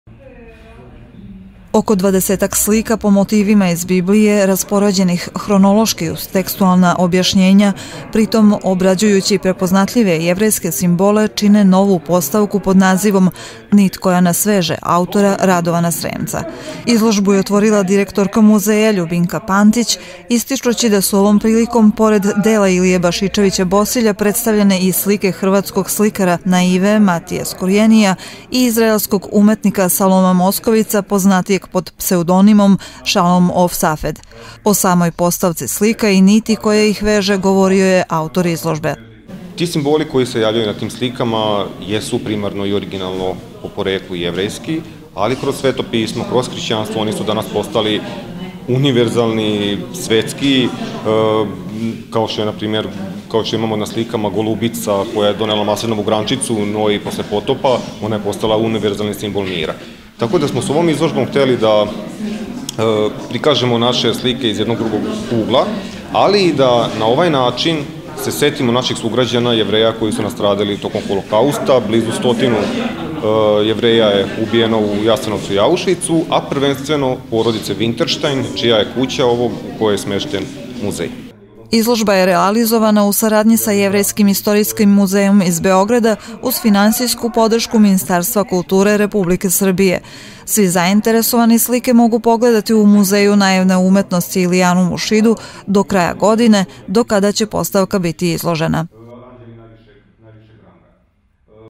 Radovan Sremac - Izložba
U Muzeju naivne umetnosti "Ilijanum" u Šidu  proteklog petka otvorena je izložba pod nazivom "Nit koja nas spaja" autora Radovna Sremca.Ovom izložbom obuhvaćena je tema  jevrejskih motiva na umetničkim delima iz fonda Muzeja naivne umetnosti "Ilijanum"